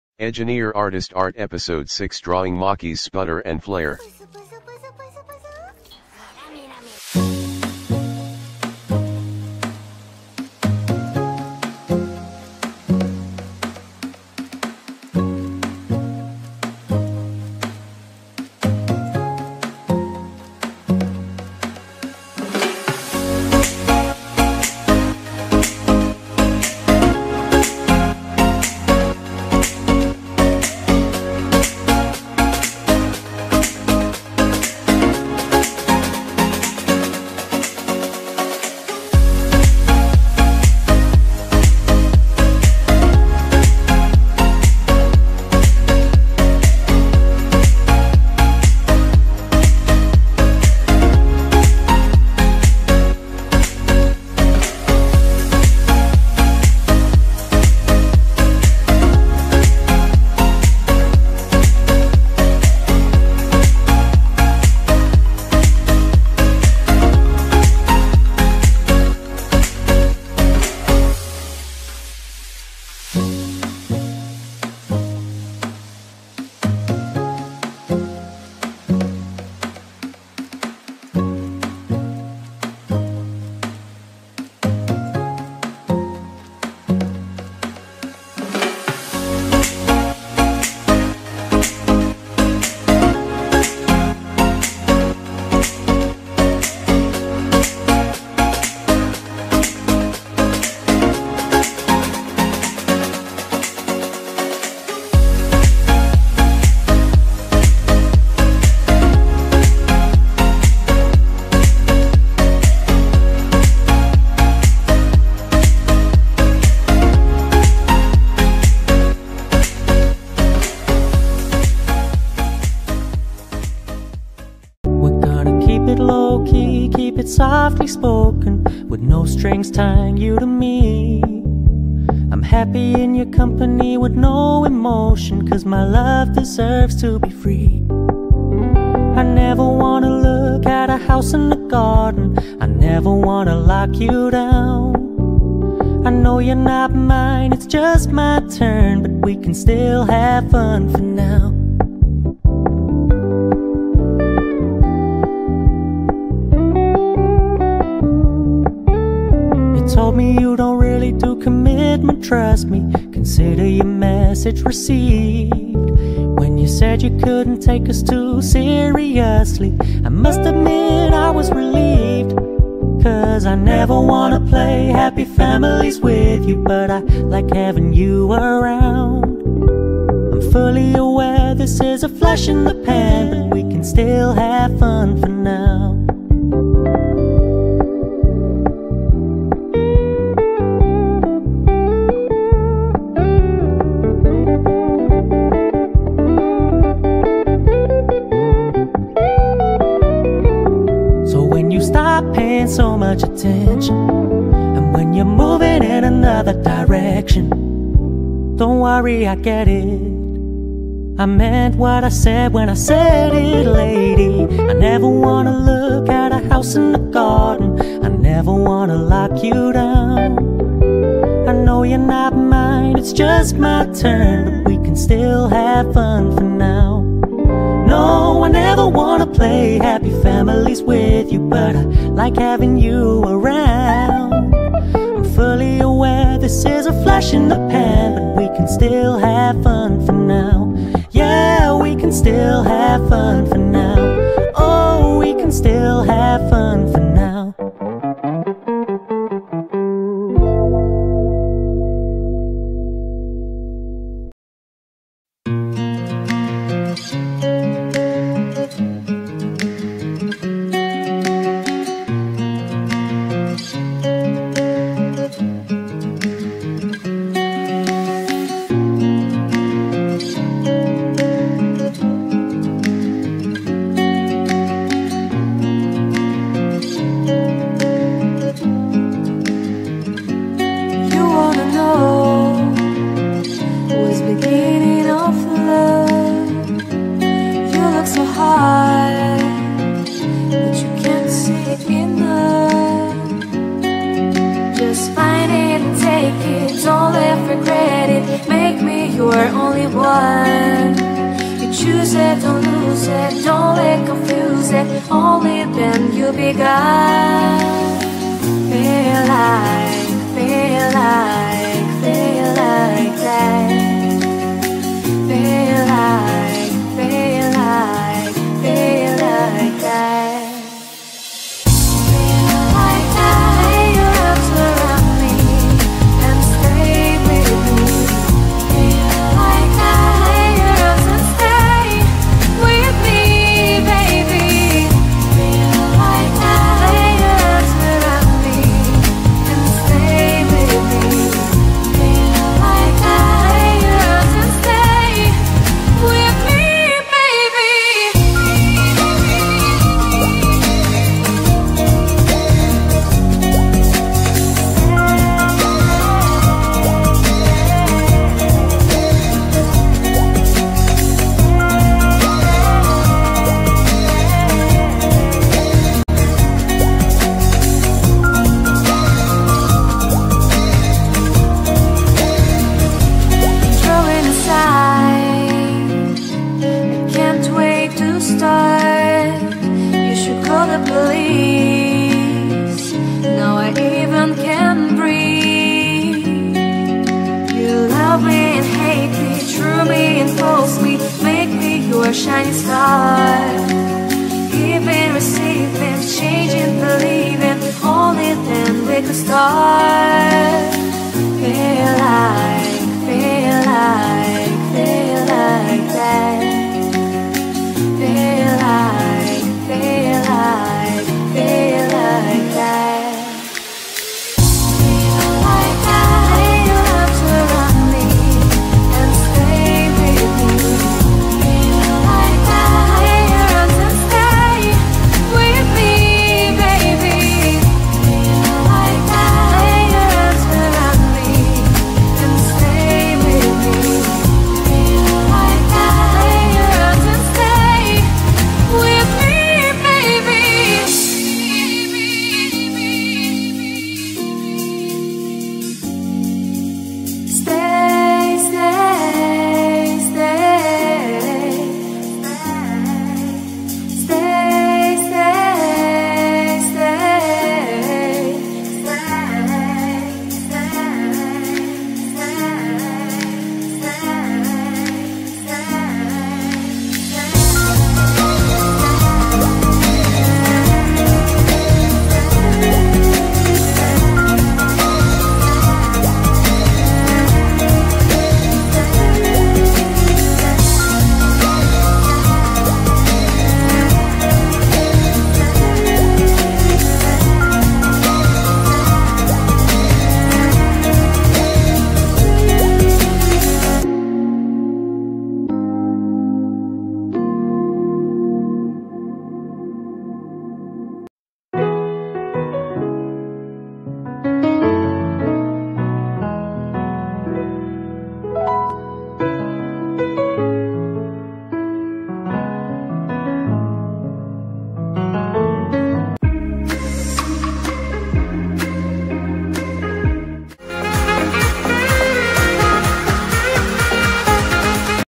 Sputter and Flare (Fire Force) in Water Color Speed Painting |Art EP6
I have always been a fan of cute anime  creatures. To cute rimuru sqlime form, kuribo and more. As I continue watching preface, I always notice Maki Oze cute  flames. Then I decided to draw them and ended up making a new art video. Here it is!❤

ɴᴇᴡ ᴀʀᴛᴡᴏʀᴋ & ᴀʀᴛ ᴠɪᴅᴇᴏ

Sputter and Flare (Fire Force) in Water Color Speed Painting |Art EP6|🎨Using 𝗦𝗲𝗮𝗺𝗶 𝗔𝗿𝘁 𝟳𝟮 𝗦𝗲𝘁 𝗪𝗖 for my page 6 of 4"×7" Potentate WC Journal📔.

On Instagram, tag @mbb_music